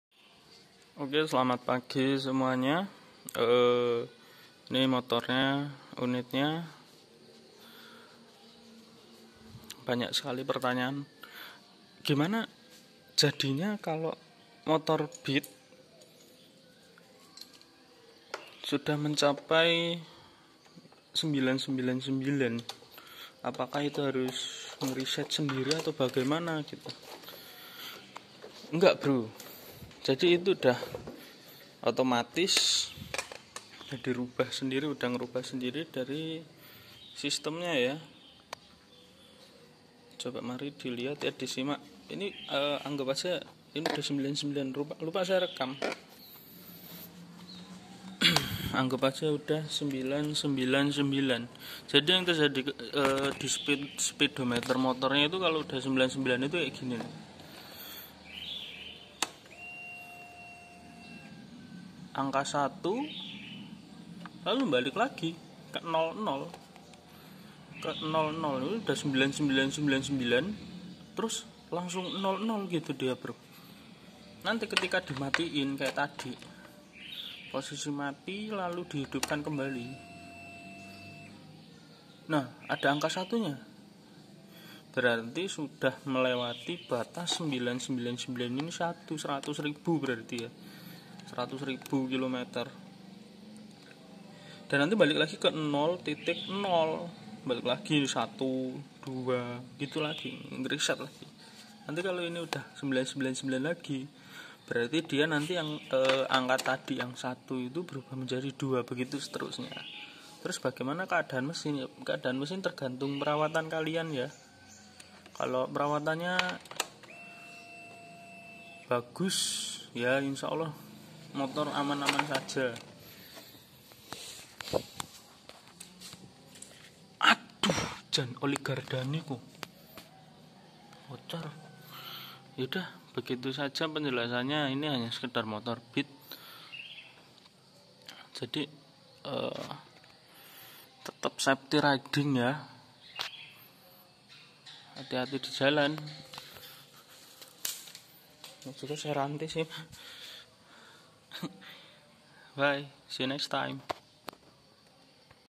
Inilah Yang Terjadi Ketika Motor Beat Mencapai Km999999 ?
Sedikit berbagi pengalaman, bagaimana jika km di motor beat sudah mencapai angka 99999 apakah harus reset manual ? Apa yang terjadi ?

Simak penjelasannya

Tengkyu

Terimakasih untuk kalian yang udah support channel ini supaya terus berkembang.
Jangan lupa untuk tetap berkendara dengan aman dan santuy